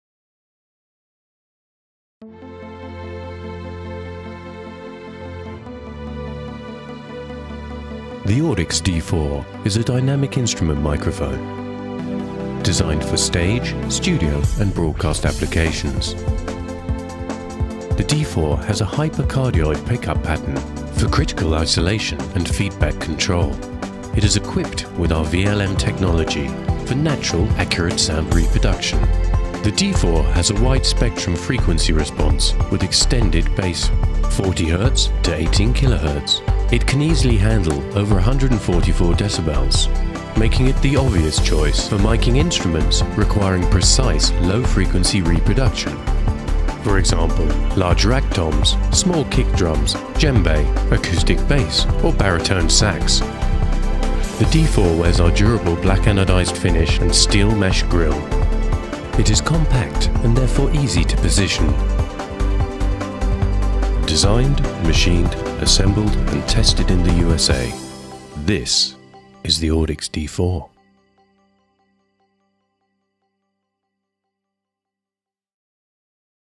Audix D4 Drum Microphone1
Con una respuesta de frecuencia amplia de 40 Hz - 18 kHz y la capacidad de manejar SPL de más de 144 dB, el micrófono de instrumento dinámico D4 es una excelente opción para los micrófonos que requieren una reproducción precisa de baja frecuencia, como toms de rack grandes, toms de piso, Cajón, djembe, tímpano, fondo de leslie, flauta para bajo, saxofón saxofón y barítono, trombón, bajo acústico, gabinetes para bajos y bombitas pequeñas.

La D4 es ligera, compacta y fácil de colocar. Utilizado para aplicaciones de escenario, estudio y transmisión, el micrófono de instrumento dinámico D4 está diseñado con un patrón de captación hipercardioide para el control de aislamiento y retroalimentación. El hipercardioide D4 también está equipado con un diafragma VLM ™ para una reproducción de sonido natural y precisa.